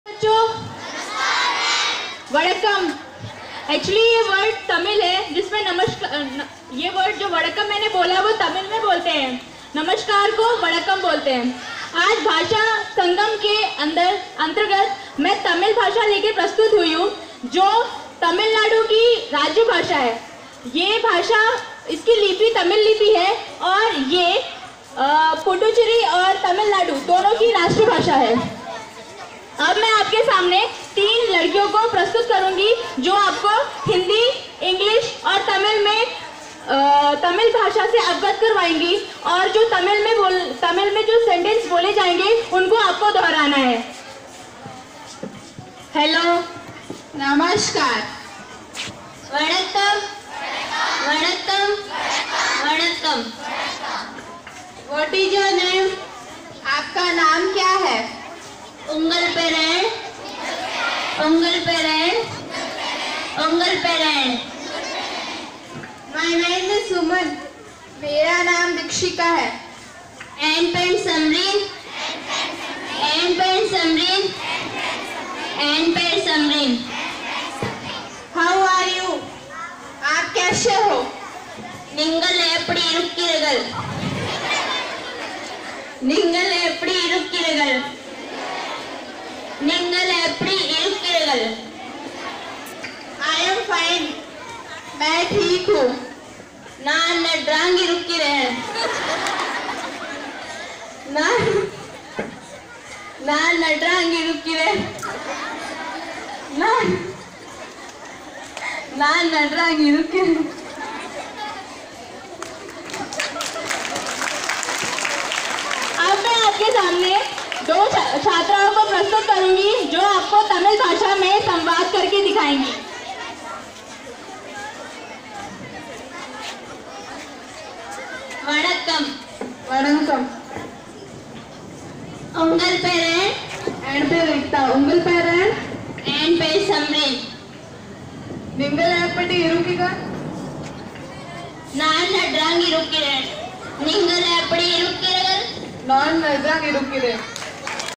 Tamil Language (Bhasha sangam)
Practicing Tamil language under thorough guidance of teacher with the help of charts  under Bhasha sangam.